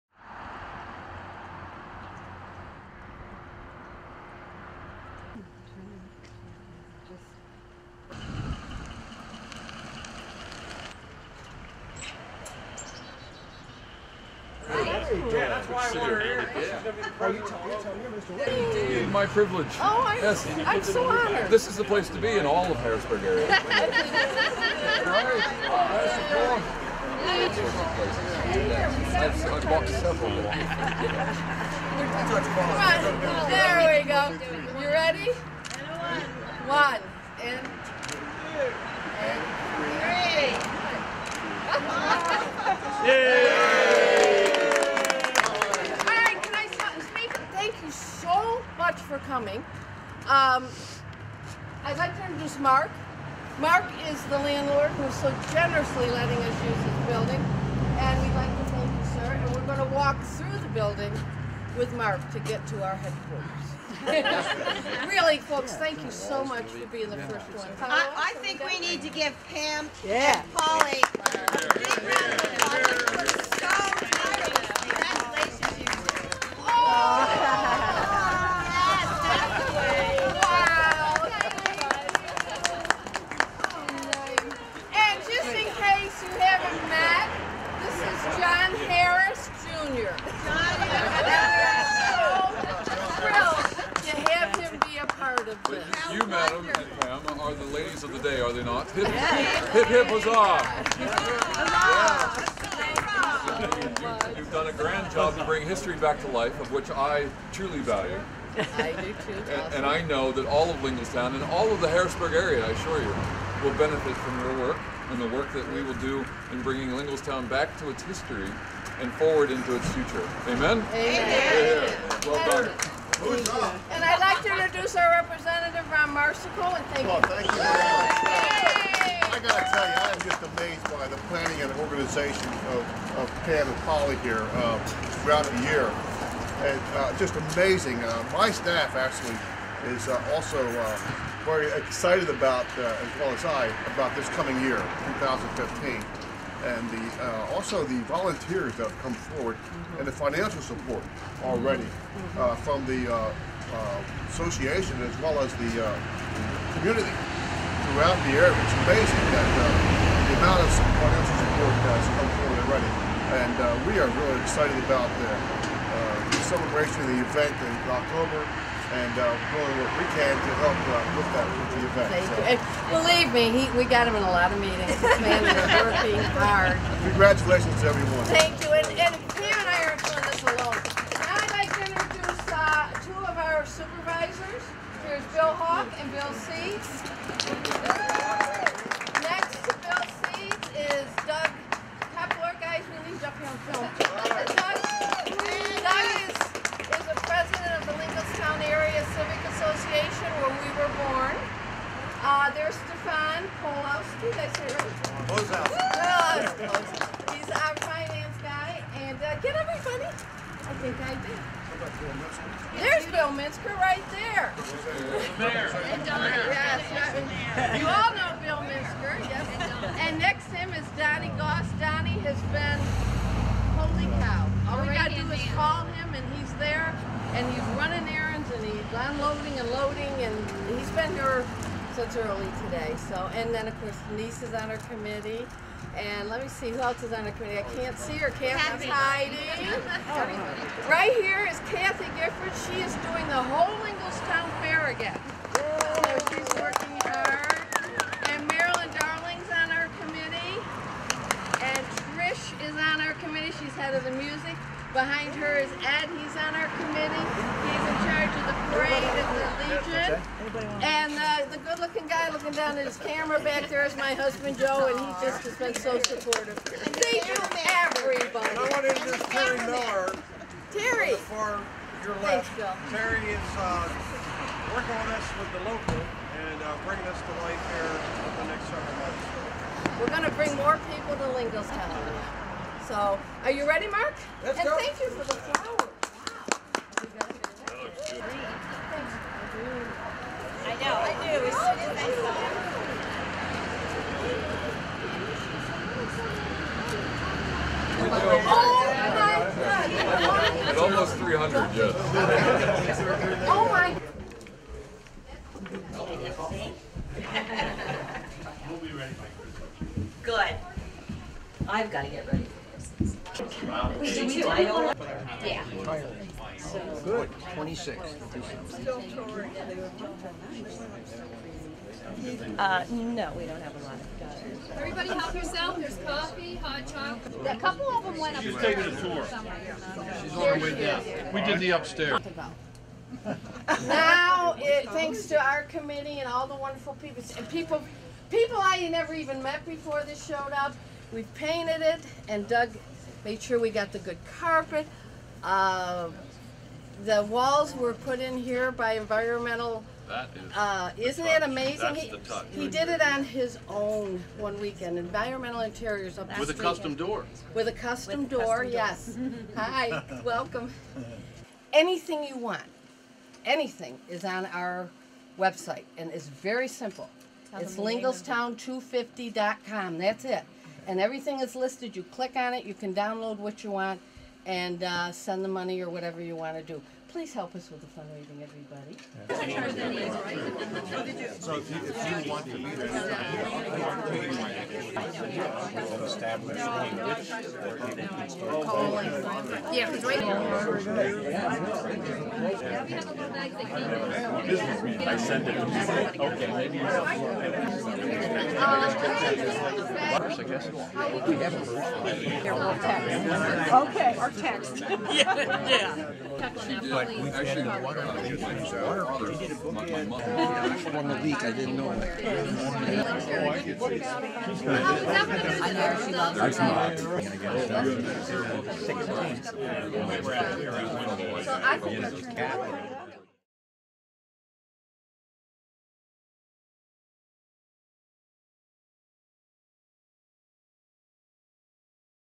Linglestown's 250th Anniversary Headquarters Grand Opening
Location: 5943 Linglestown Road.(at rear of building).
Hours open: Thursdays 4pm-7pm, Saturdays 10am-2pm.